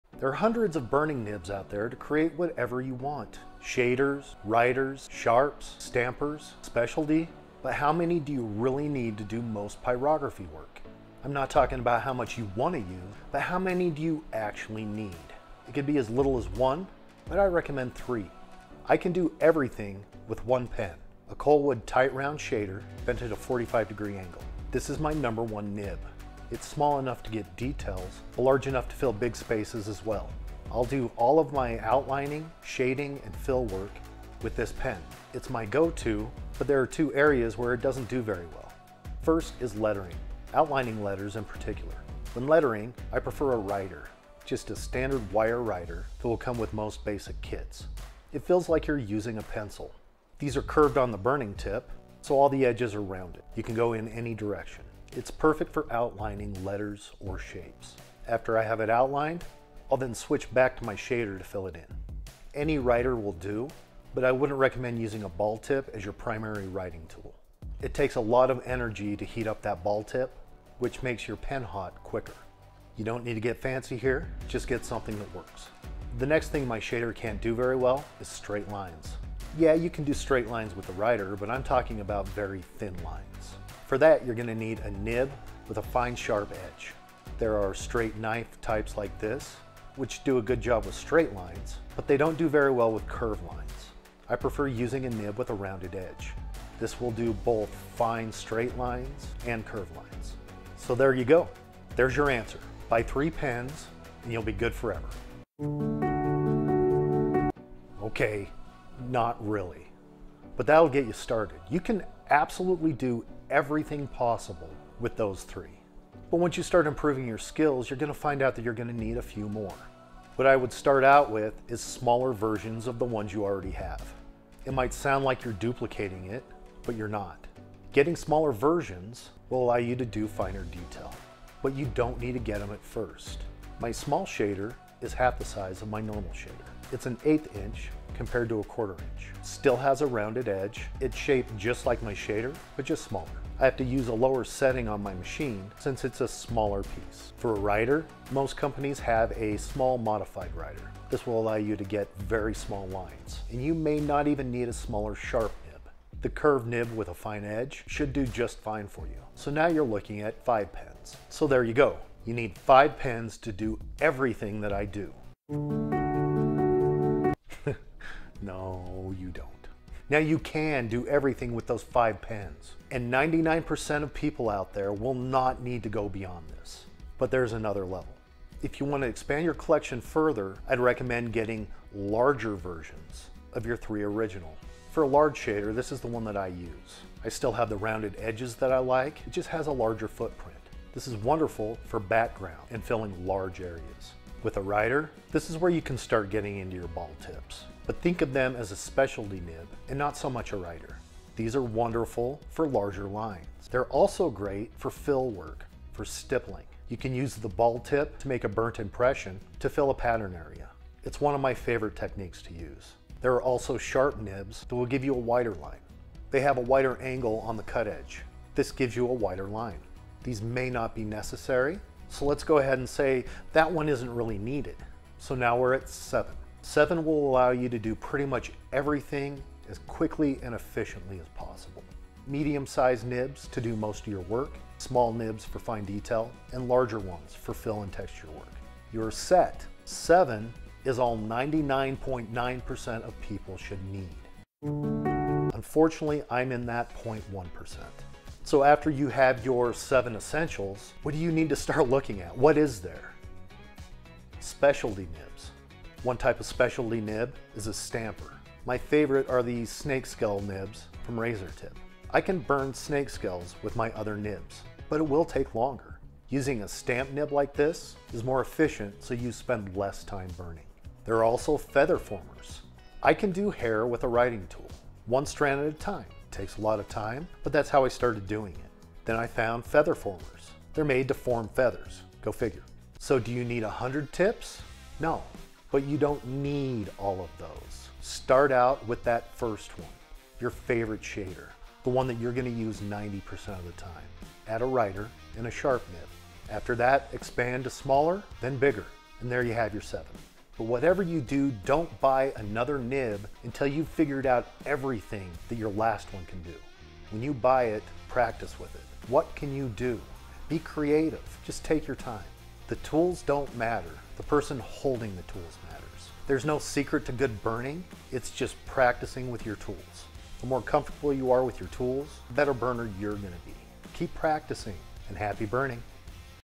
How Many Pyrography Pens Do You Really Need?
Dirk the Pyrographer walks you through what pens you need to do every pyrography technique under the sun.